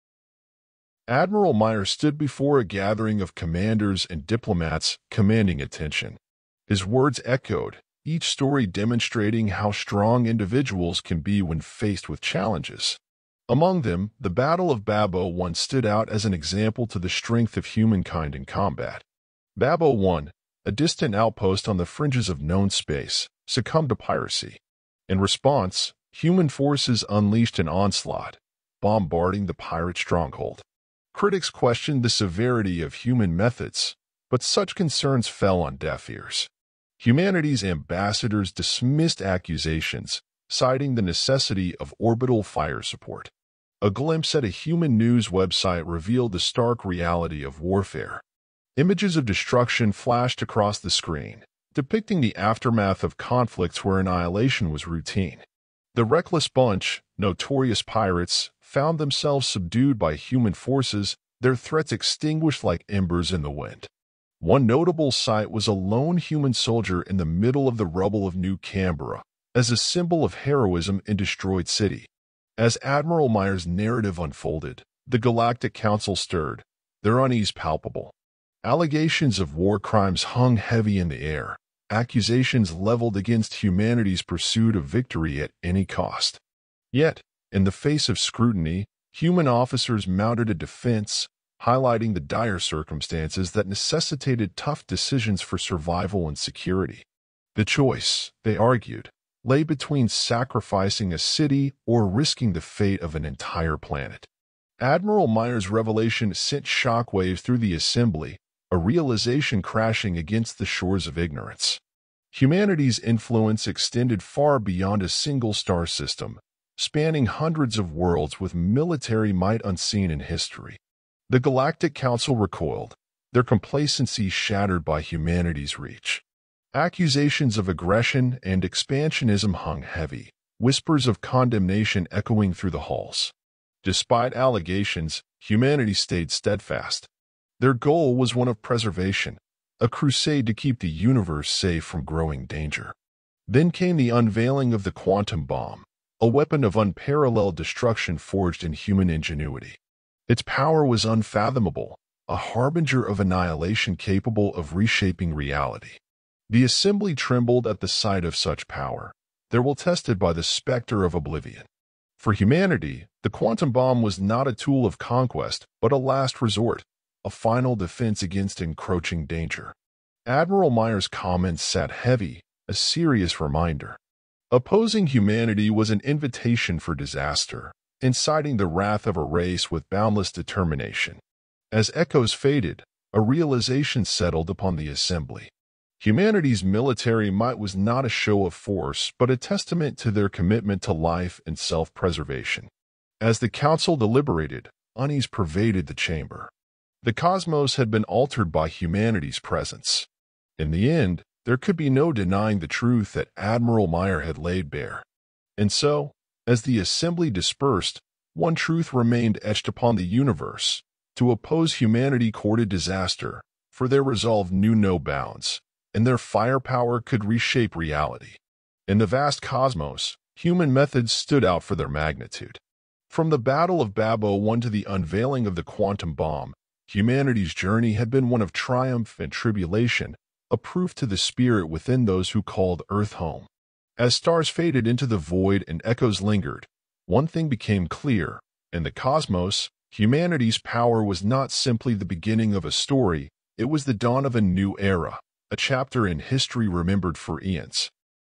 HUMANS, Don't Have LIMITS I HFY I A Short Sci-Fi Story
Admiral Myer addressing a gathering of commanders and diplomats, recounting humanity's response to piracy and threats in the vastness of space.
From the Battle of Babbo 1 to the unveiling of a quantum bomb, the narrative showcases humanity's resolve and ingenuity in facing existential challenges. Despite allegations of aggression and war crimes, Admiral Myer emphasizes humanity's commitment to preservation and safety, culminating in the realization that opposing humanity invites disaster.
The story paints a picture of humanity's rise to prominence in the cosmos, marked by triumphs, sacrifices, and the unwavering determination of those who call Earth home.

Do not use content without my written approval.